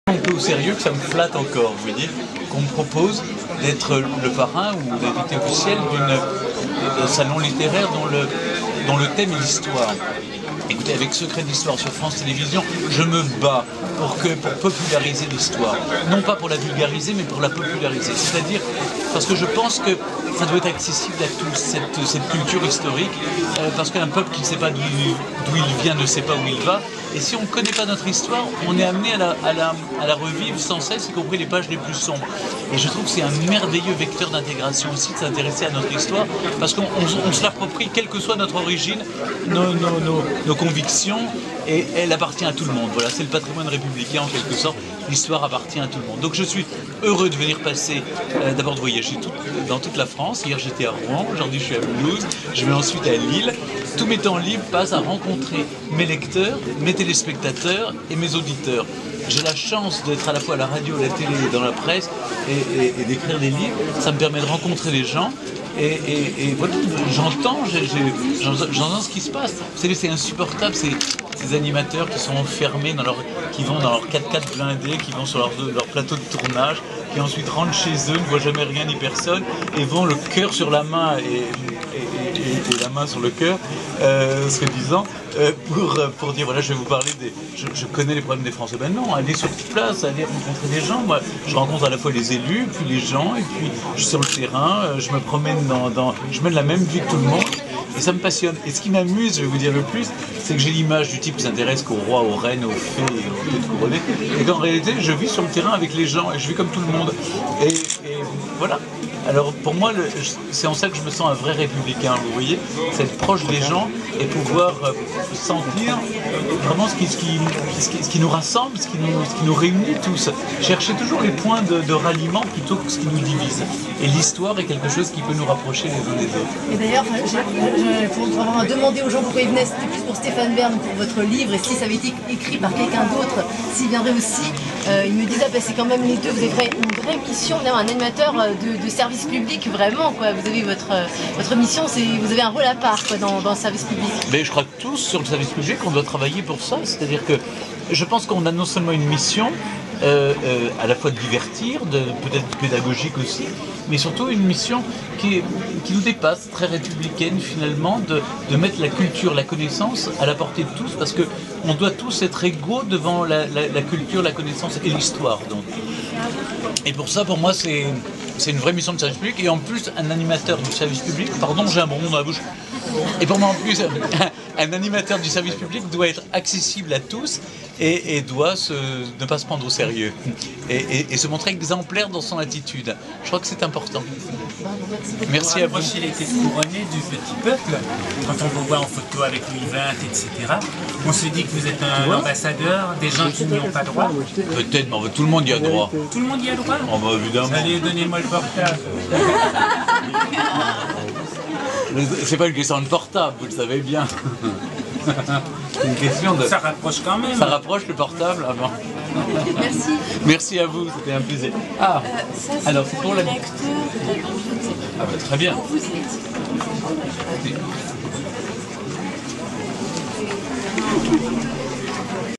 stéphane bern.mp4
Stéphane Bern, en pleine séance de dédicaces à la bibliothèque municipale devant une foule presque exclusivement féminine

Un sourire royal
Elles sont venues... pour lui. Exclusivement. Pour Stéphane Bern, parrain de « Tout Mulhouse lit ». « Pour sa voix, pour son sourire », avoue Corrine une lectrice assidue. Et personne n'a été déçu.

Article paru dans l'édition du
Dimanche 24 Octobre 2010 DNA